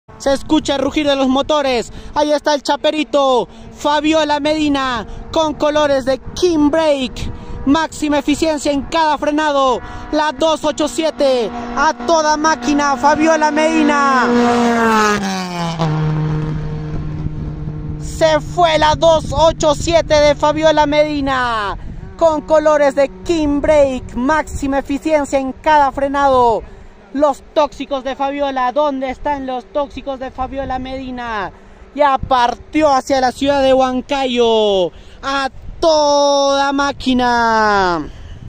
🏁 Fabiola Medina PE1 Canta La Cima / Rally Caminos del Inca 2024 / King Brake Perú 🇵🇪
Rally Caminos del Inca 2024
Realizamos recopilaciones de videos.
Solo disfruta el video todos somos apacionados por los coches

Somos apasionados por el Rally 🏁
WRC
Dakar
Caminos del Inca (Perú)